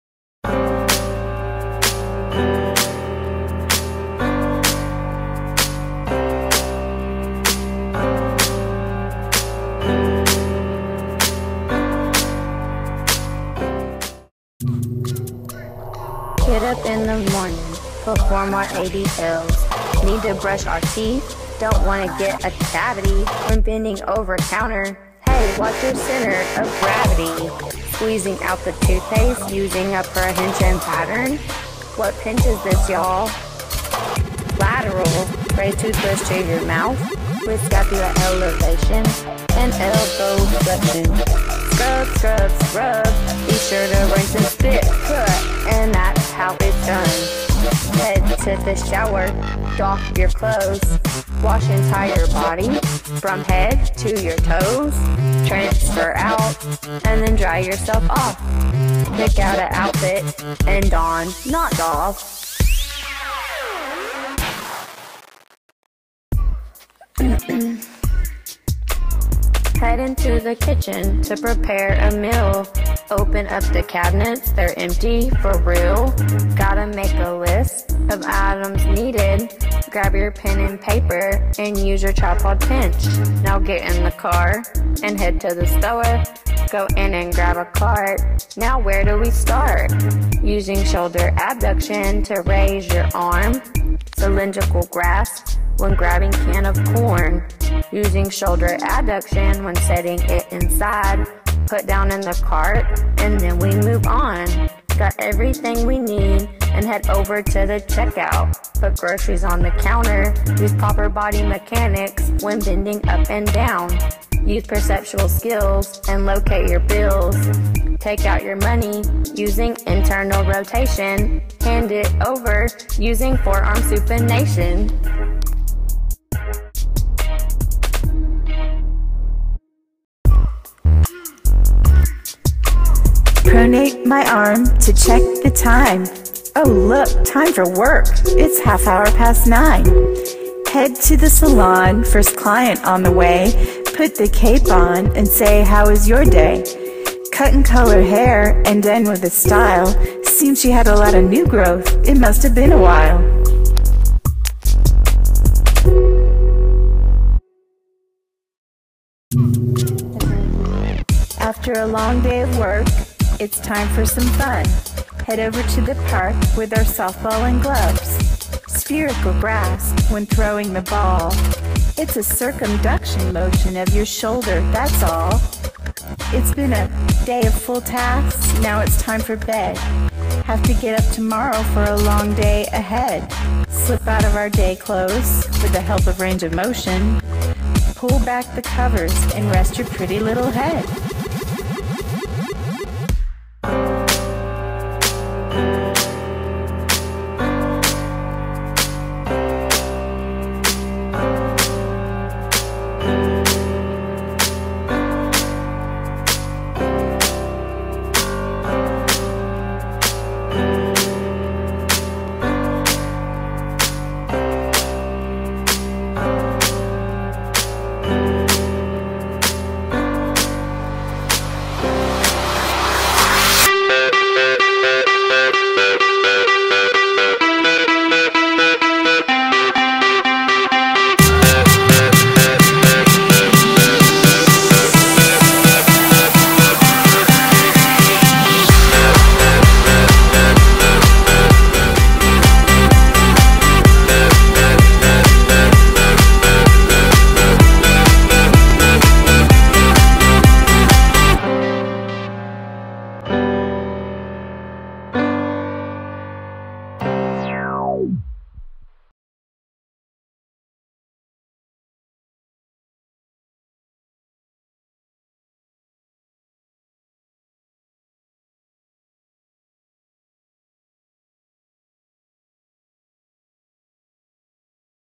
MurrayNitche_MStudy
Fun with OTA terminology. Motion Studies in Kinesiology homework.